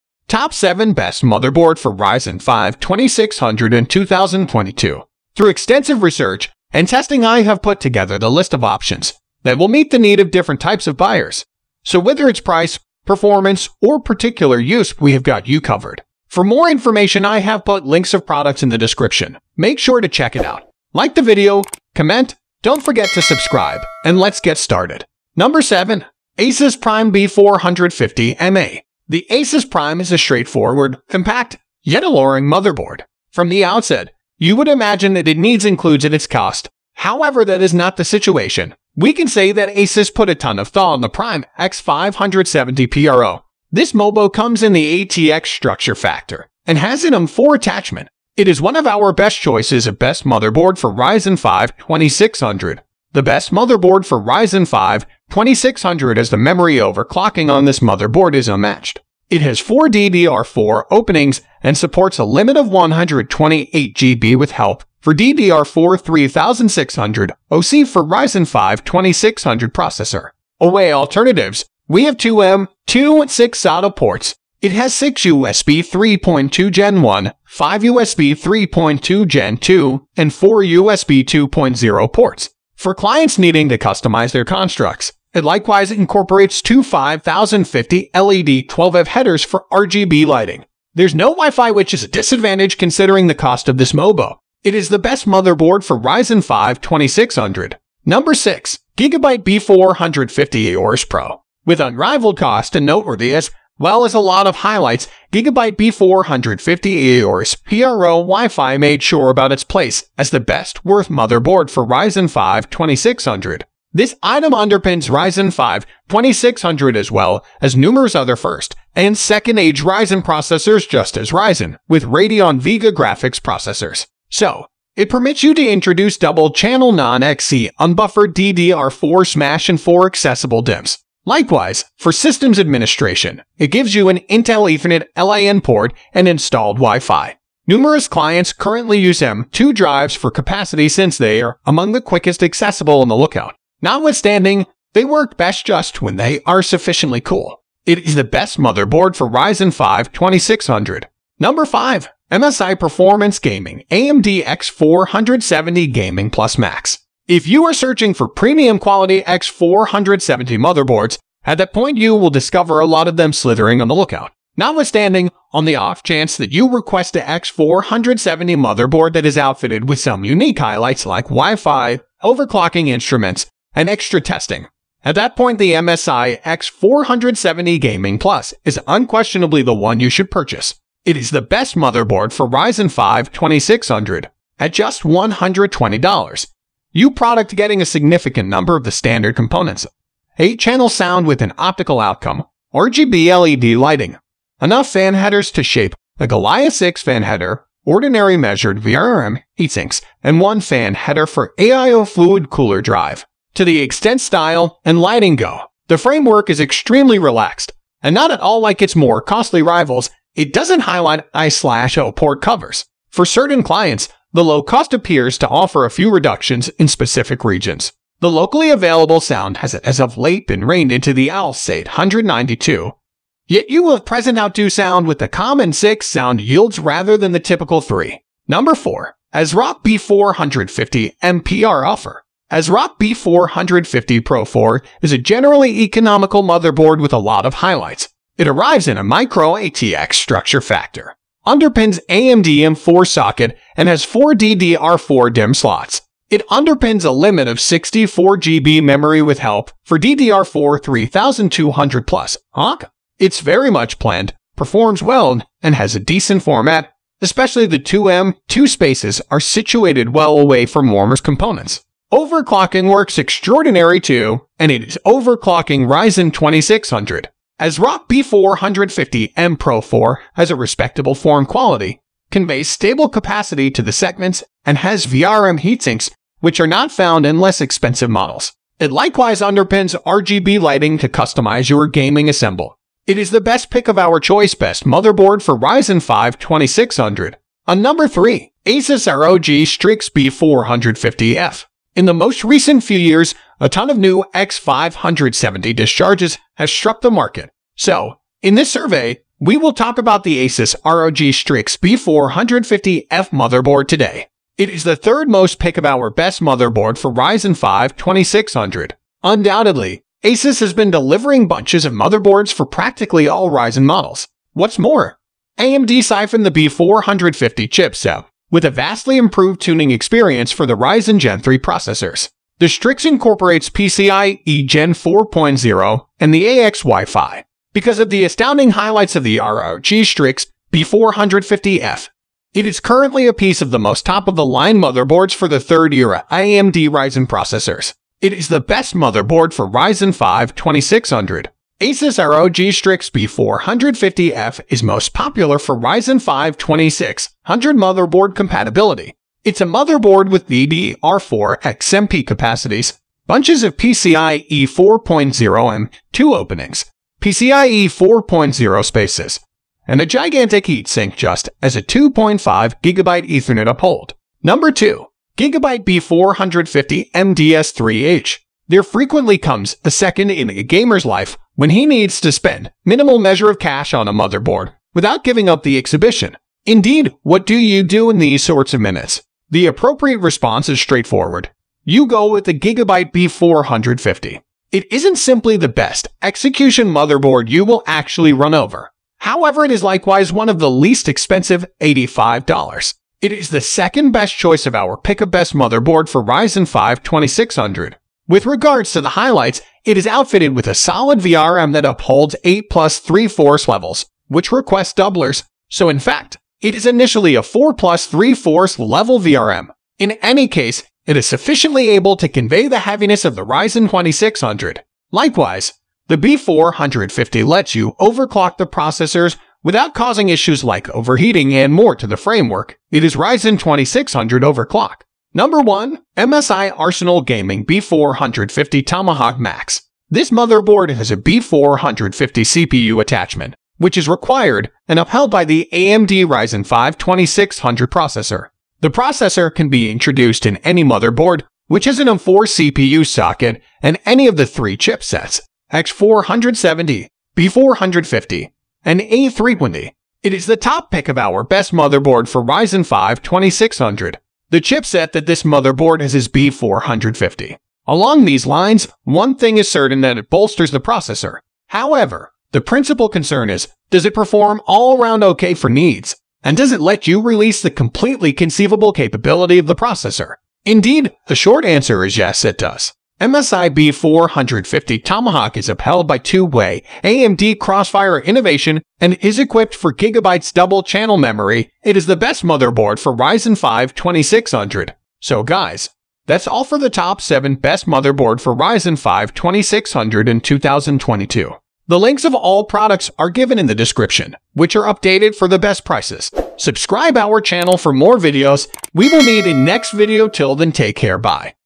Top 7 Best Motherboard For Ryzen 5 2600 In 2022
➜ Links to the Top 7 Best  Motherboard For Ryzen 5 2600 we listed in this video:

►Links►

➜ 7 - ASUS RPIME B550M-A

➜ 6 - GIGABYTE  B550 AORUS PRO

➜ 5 - MSI PARFORMNCE GAMING AMD X470 GAMING PLUS

➜ 4 - AS ROCK B550M  PRO 4

➜ 3 - ASUS ROG STRIX B450-F

➜ 2 - GIGABYTE B450M DS3H

➜ 1 - MSI ARSENAL GAMING  B450  TOMAHAWK MAX